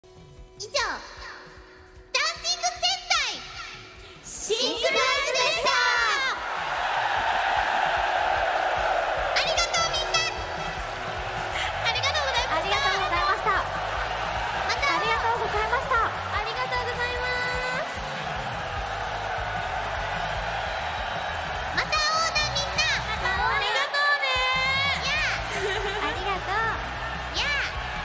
5yncri5e! Super Sentai Pose!
Nakonako y su guardería posando como en los tokusatsu.